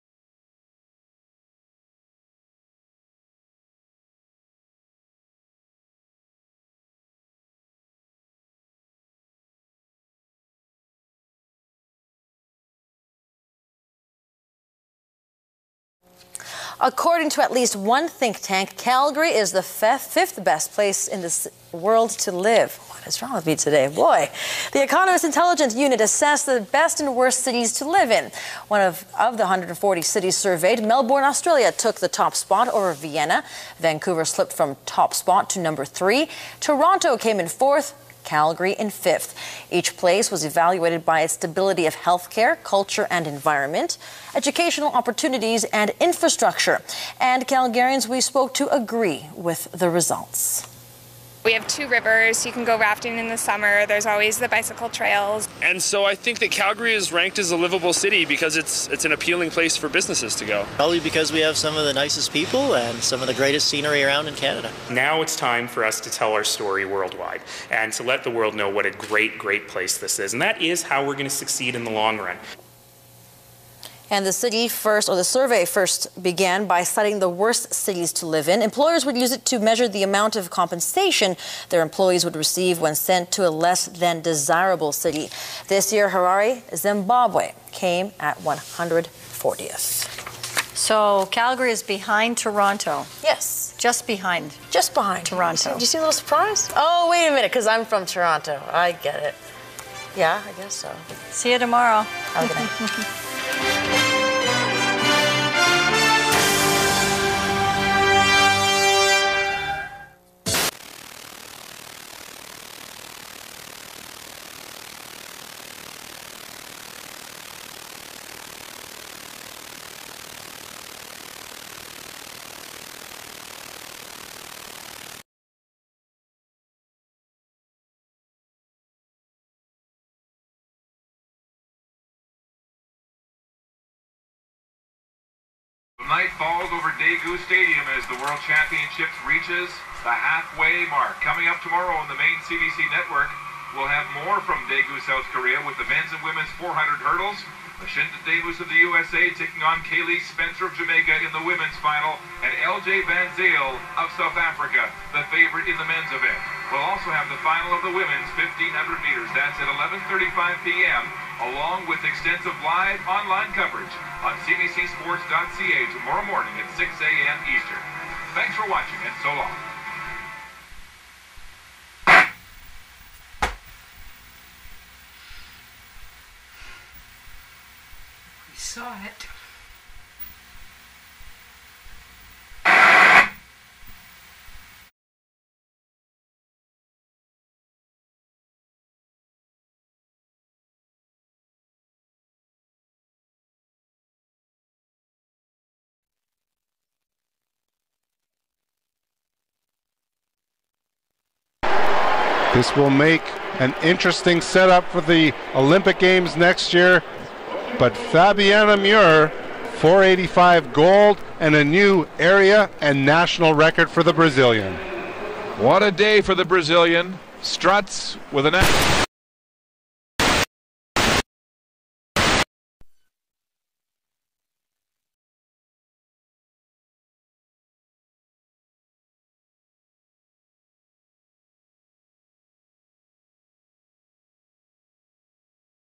Episode 130 - Analog TV Shutdown To Digital TV | (August 31 & September 01 & 04, 2011)
Note: CFCN-CBOT-TV Channel 4 & CBET-TV Channel 9 We Are OFF-AIR On Analog TV Channel Is Shutdown By Canada:

Courtesy: thepasswordisfail, ManifestationTV & operationmeatspin